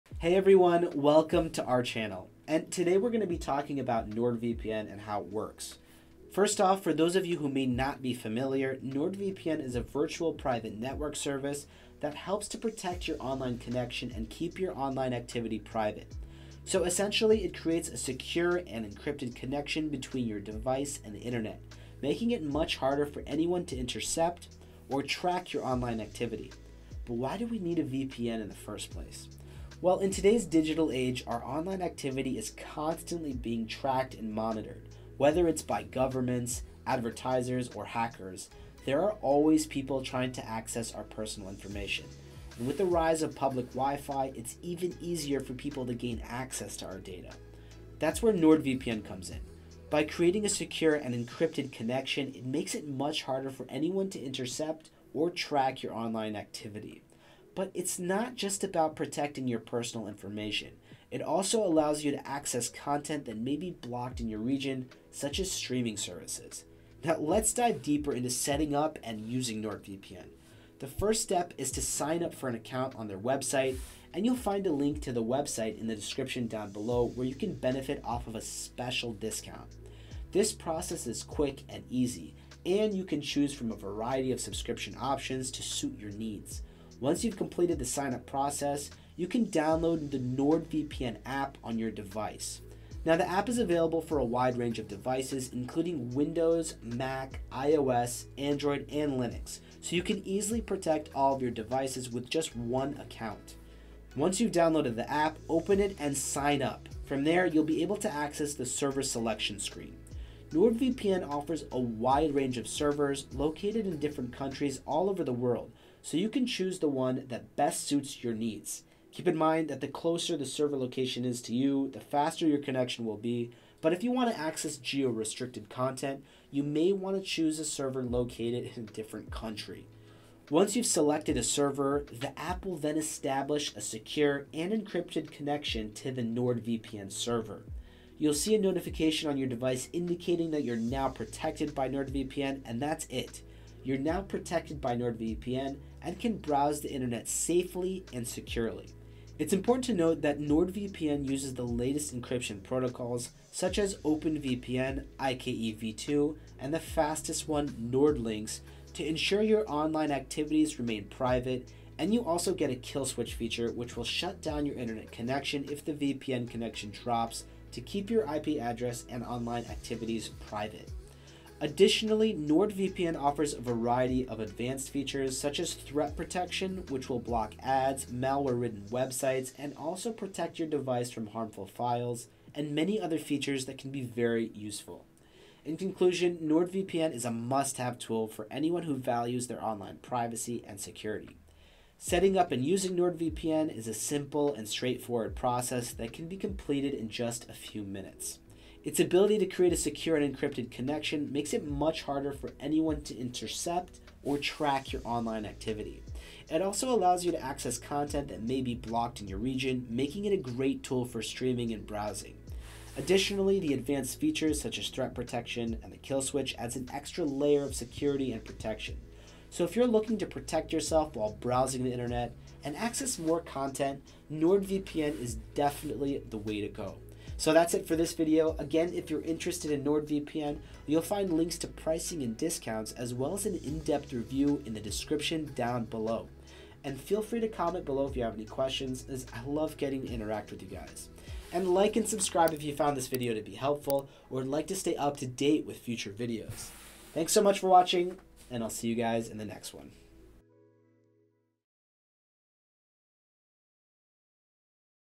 How Does NordVPN Work? Set up & Use Tutorial
How does NordVPN work? Plus how to set it up & use it. NordVPN is a powerful VPN service that helps you stay secure online. Learn how it works, plus how to set it up and use it!

Timestamp:

00:00 - Intro
00:10 - What is VPN?
00:29 - Why we need VPN?
01:09 - Setting up & using the VPN
02:30 - Security features
02:54 - Advanced features
03:06 - Conclusion
03:47 - Outro

Today, we're diving into Nord VPN, a virtual private network service designed to safeguard your online connection and privacy. In an era where online surveillance is rampant, Nord VPN encrypts your internet traffic, making it challenging for anyone to monitor or intercept your activities. Beyond privacy protection, Nord VPN enables access to geo-blocked content, enhancing your browsing experience. Let's explore how Nord VPN works and its benefits.

Setting Up and Using Nord VPN

To get started with Nord VPN, sign up on their website, and choose a server, especially if you aim to access geo-restricted content. Once connected to a Nord VPN server, your device establishes a secure and encrypted connection, ensuring your online safety. Utilizing advanced encryption protocols like OpenVPN and IKEv2, Nord VPN prioritizes user privacy. Additionally, features like the kill switch prevent data leaks in case of a VPN connection drop. Nord VPN also offers advanced functionalities like threat protection, enhancing your overall online security.

In conclusion, Nord VPN is indispensable for individuals prioritizing online privacy and security. Its straightforward setup process and robust encryption make browsing safe and secure. Moreover, Nord VPN's ability to bypass geo-blocks and protect against threats elevates its utility. Whether you're streaming content or simply browsing, Nord VPN offers comprehensive protection.

If you're looking to enhance your online security and access restricted content, Nord VPN is a top choice. Check the description for pricing, discounts, and further details.

Hope you enjoyed my How Does NordVPN Work? Set up & Use Tutorial Video.